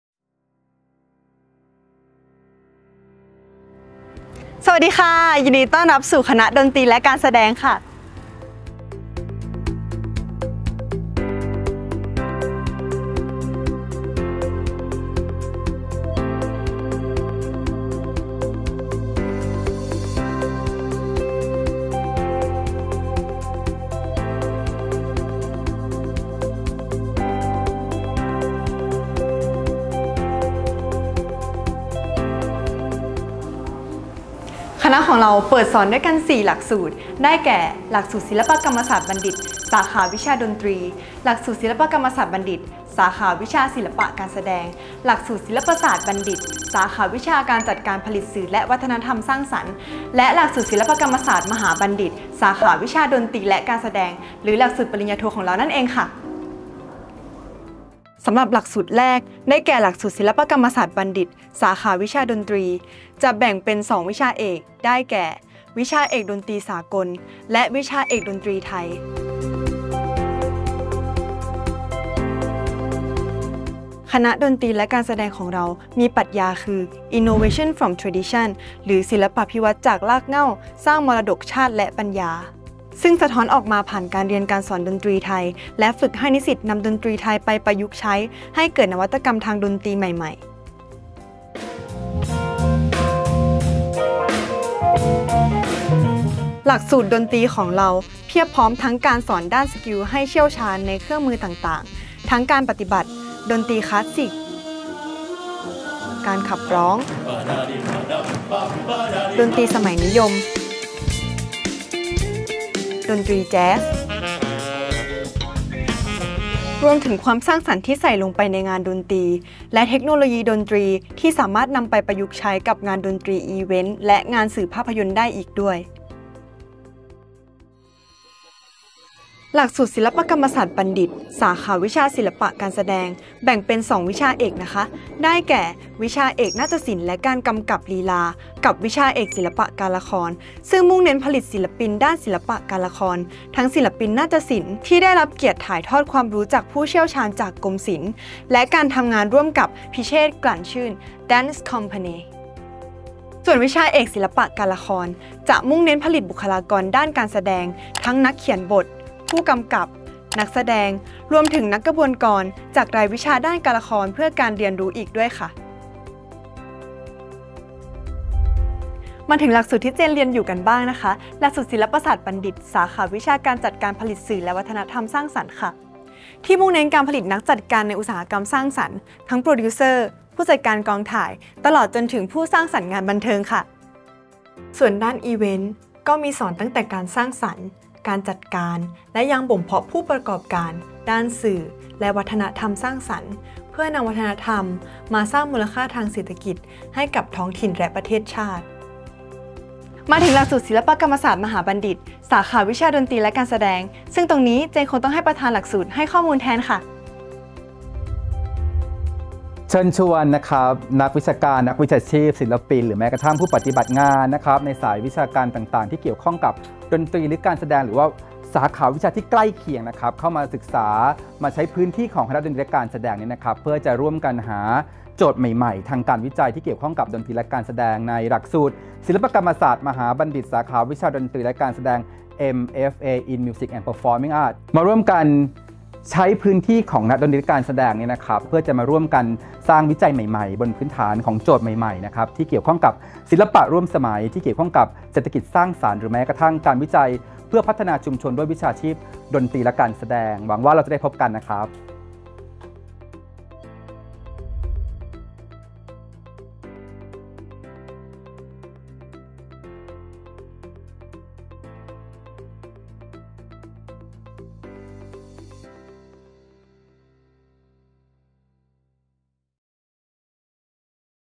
คณะดนตรีและการแสดงของเรามีหลักสูตรอะไรบ้างนะ?! - 20/11/2023
คณะดนตรีและการแสดงของเรามีหลักสูตรอะไรบ้างนะ?!
วันนี้พี่ PINK พาพี่ เจนนิเฟอร์ พาร์คิน พี่นิสิตในคณะฯ
มาเล่าให้น้อง ๆ ฟังค่ะ!

📔💟 TCAS67 - 1 PORTFOLIO 📔💟
คณะดนตรีและการแสดง มหาวิทยาลัยบูรพา
เหลือเวลาสมัคร / ส่งพอร์ท อีกเพียง 1 สัปดาห์!!

รายละเอียดแบบสรุปมาให้แล้ว
(โครงการ 👉 "รับตรงคณะดนตรีและการแสดง" เท่านั้น)
ดูโจทย์/ส่งคลิป/ส่งพอร์ท

Faculty of Music and Performing Arts